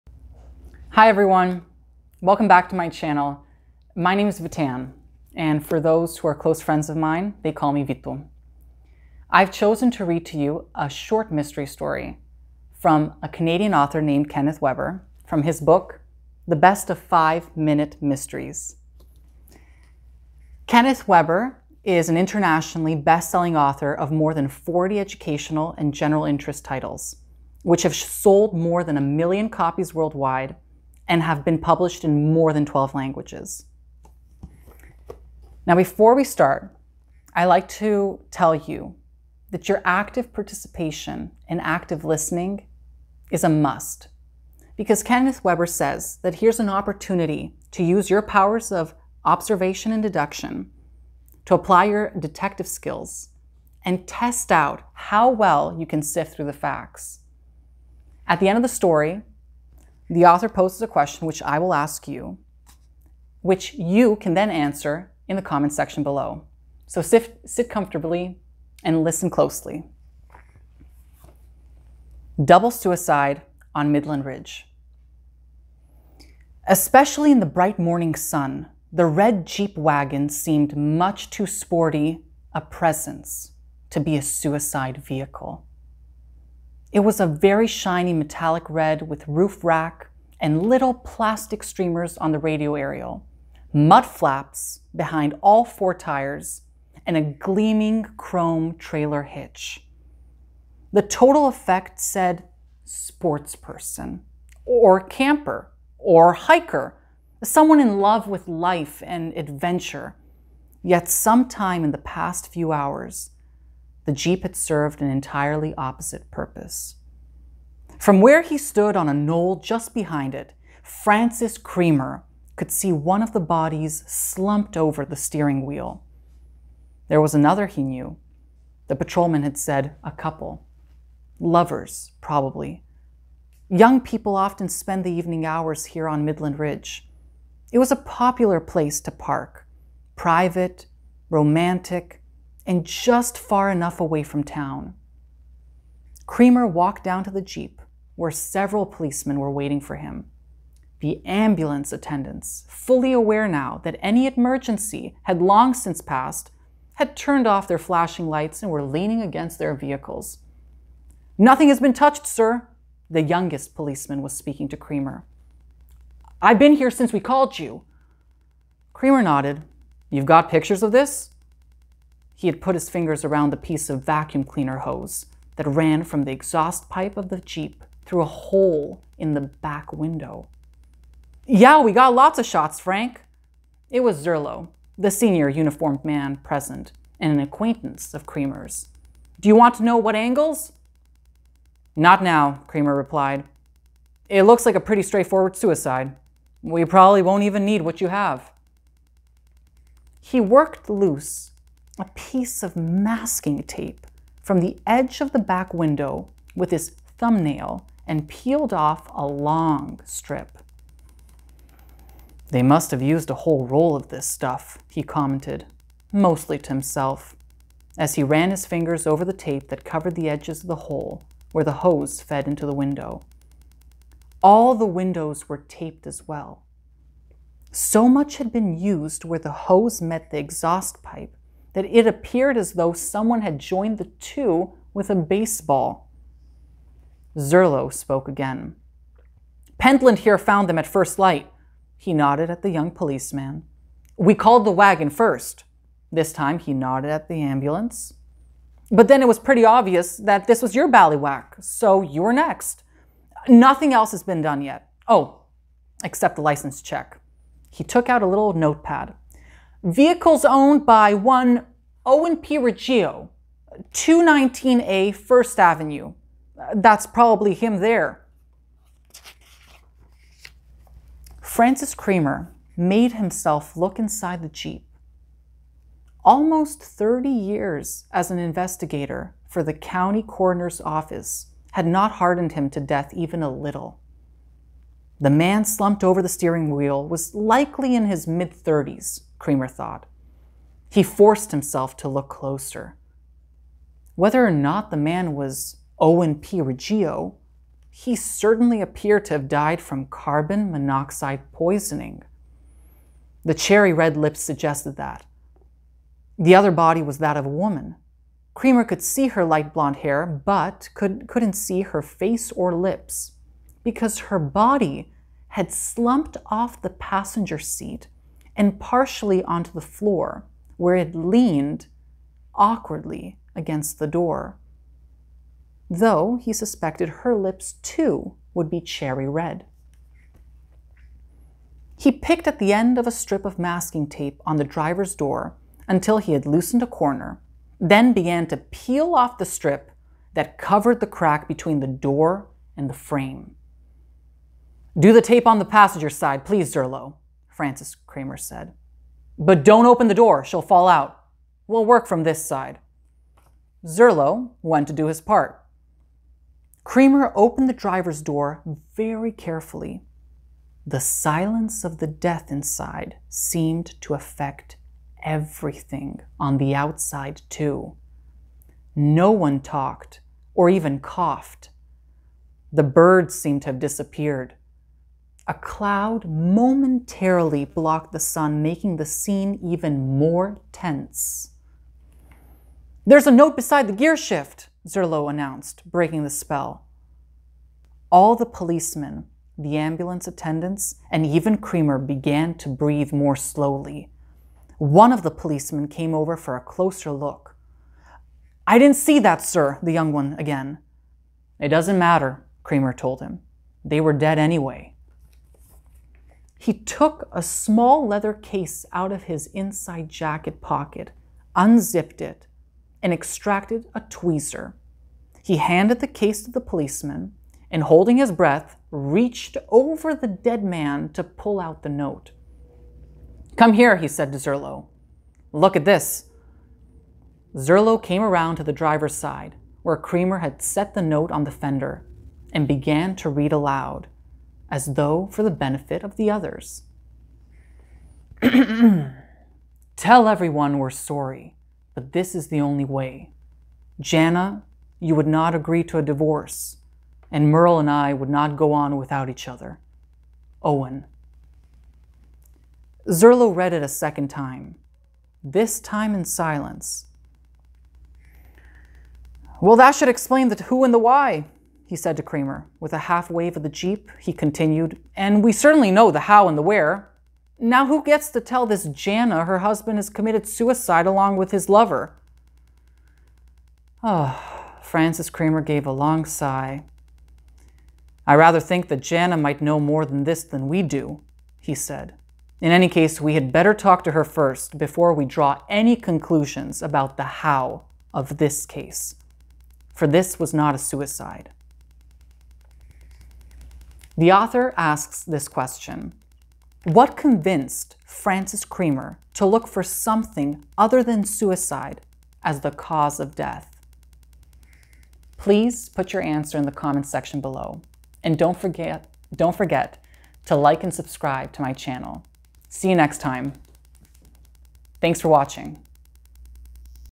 STORYtime Corner - Mystery Series: "Double Suicide on Midland Ridge" by Kenneth/Ken J. Weber
This short story is from the Canadian author, Ken Weber's book "The Best of Five-Minute Mysteries", published by Stoddart Publishing Co. Limited in 1999.

Comment your answer to the question asked at the end of the video so as to solve the mystery together.